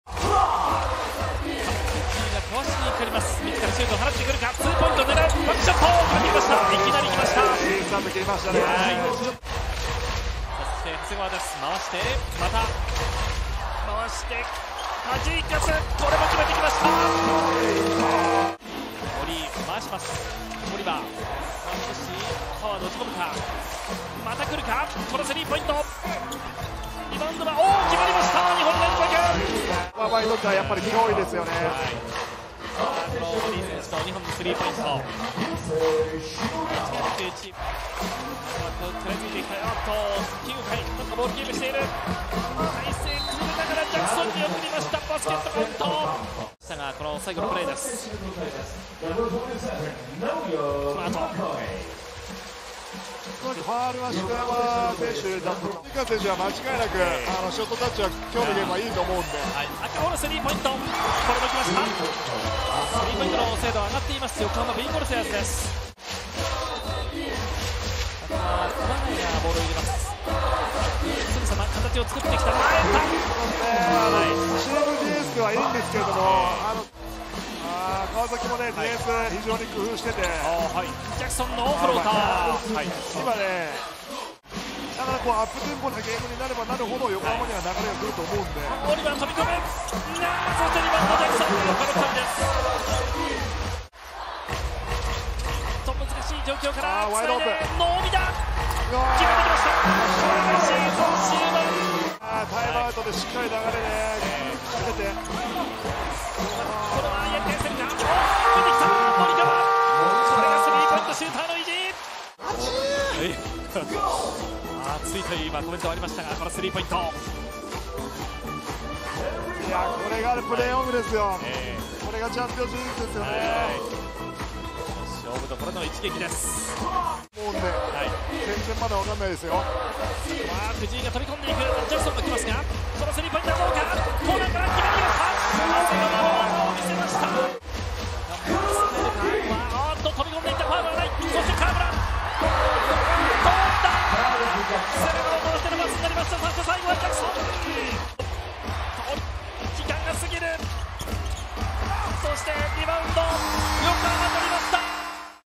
【ハイライト】川崎ブレイブサンダースvs横浜ビー・コルセアーズ｜日本生命 B.LEAGUE QUARTERFINALS 2022-23 GAME1｜05.13.2023 プロバスケ (Bリーグ)
Watch the Game Highlights from Kawasaki Brave Thunders vs. Yokohama B-Corsairs, 05/13/2023